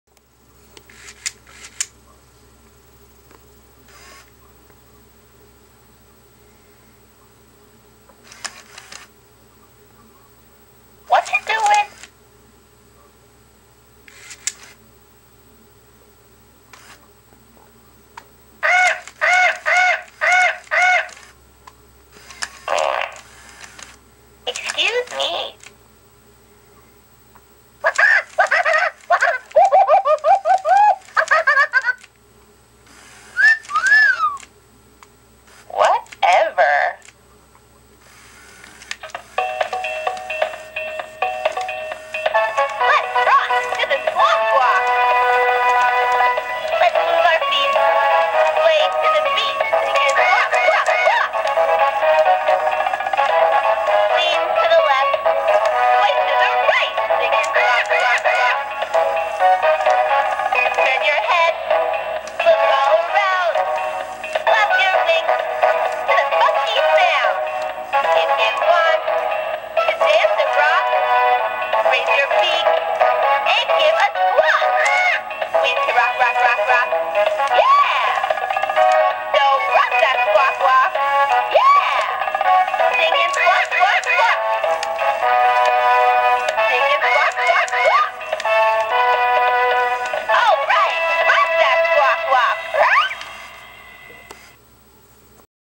Hasbro FurReal Parrot Squawkers McCaw Bird Interactive Toy + Perch + Remote
This toy is Amazing. FurReal Parrot Squawkers McCaw. Made by Hasbro. This toy does all kinds of things. Talks, Farts, SIngs, Dances. He is for sale in my eBay store. These birds are no longer made by Company. Here is a video of some of things he does.